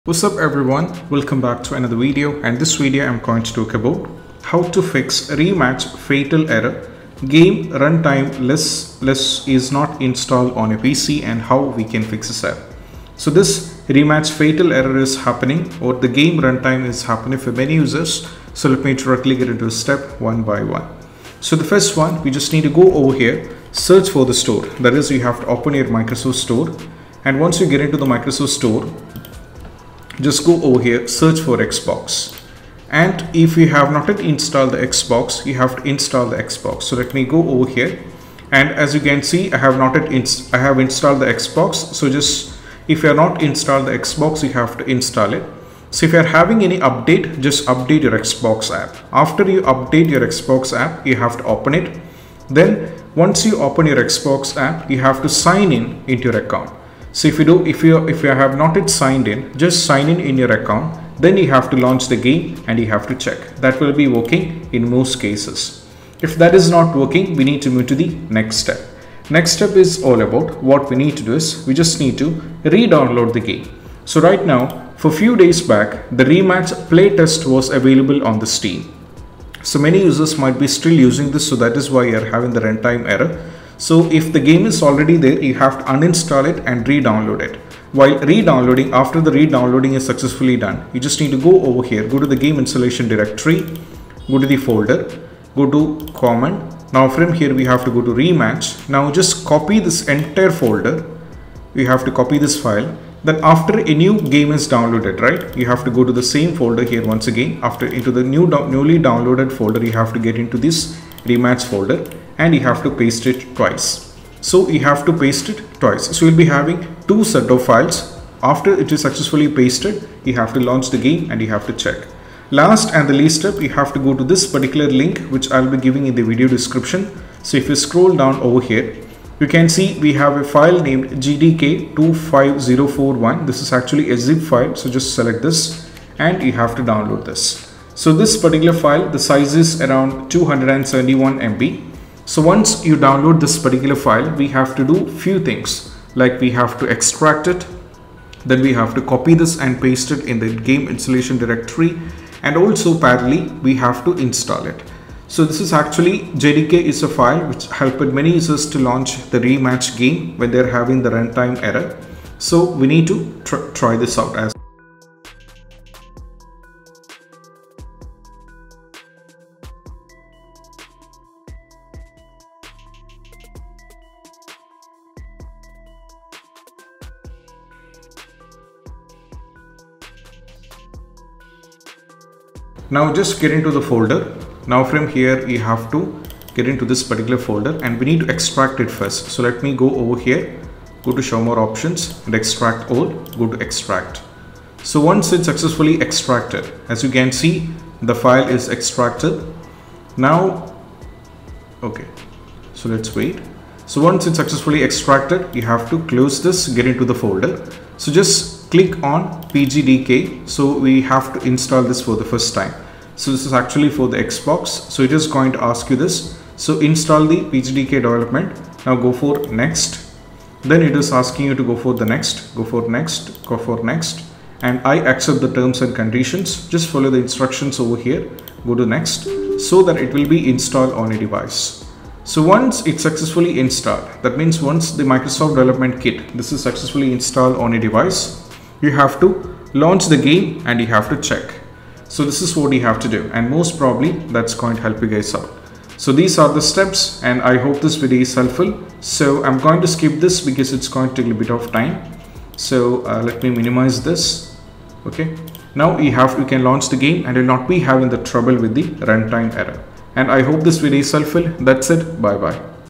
How to Fix Rematch fatal error game runtime is not installed
In this video, I will Fix the Rematch fatal error that says “Game runtime is not installed” when launching the game on your Windows PC. This video provides a complete solution to fix the crashing or not launching issue with the Rematch game.